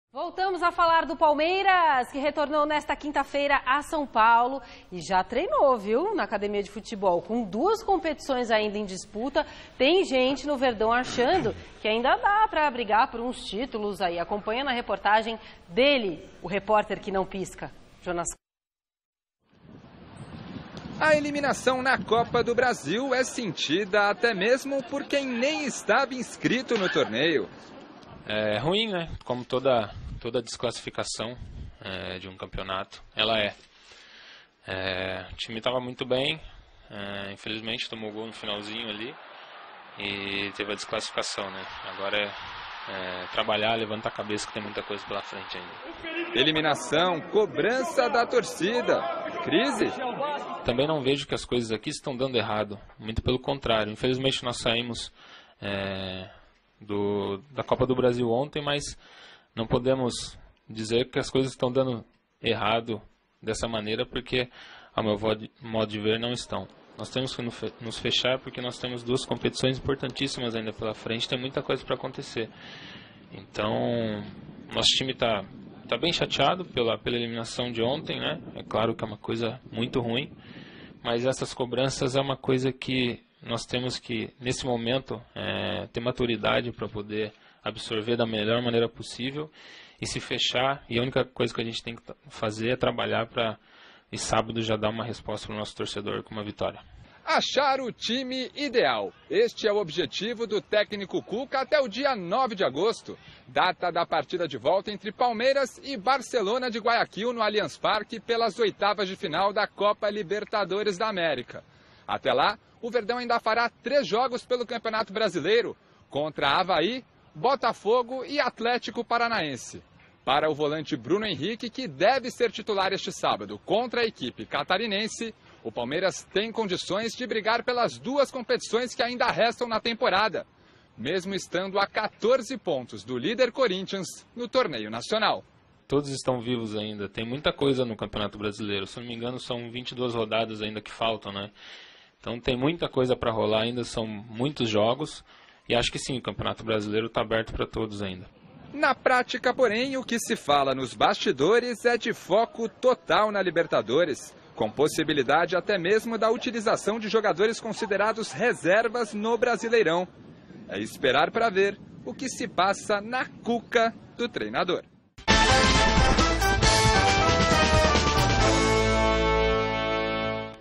Palmeiras está em crise? - Gazeta Esportiva (27/07/17)
Crise? Após eliminação na Copa do Brasil, Palmeiras volta aos treinamentos nesta quinta-feira